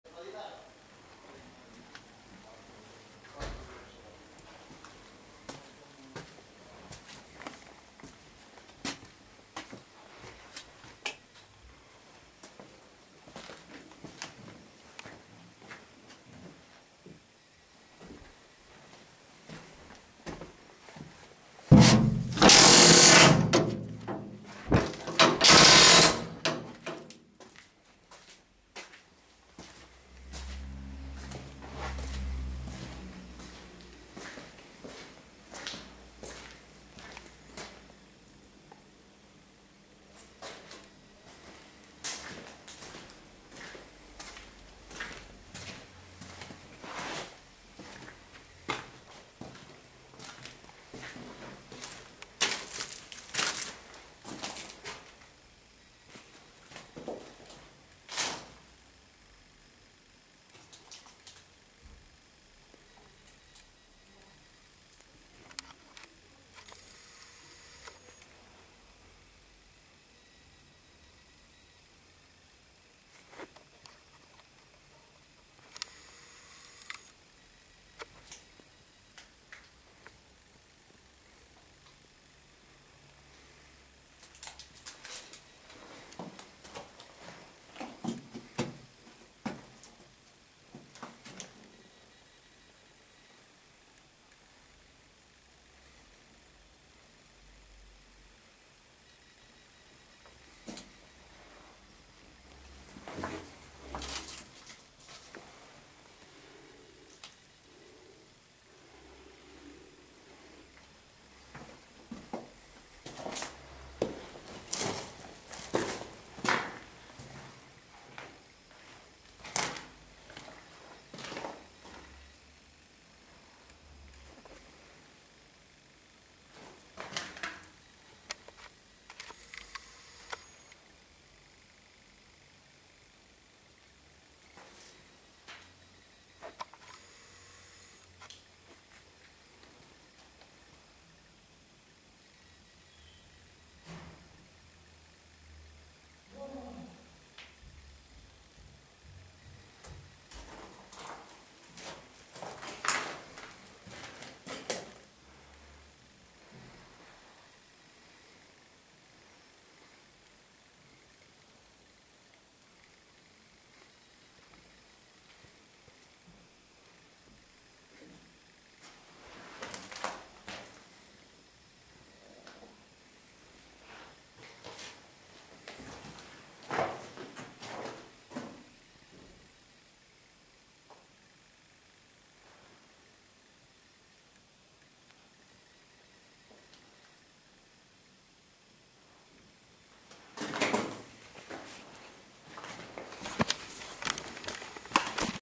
Traitement contre la mérule [ à Bruxelles ]
Un grave problème de mérule constatée dans cette maison qui est verbalisée par le Service Régional Bruxellois du Logement comme insalubre mais avec modifications.
Entre autre des dysfonctionnements électriques, d'eau ou de la présence de rats et souris en surnombre, les murs sont envahis de mérules.
N° vert : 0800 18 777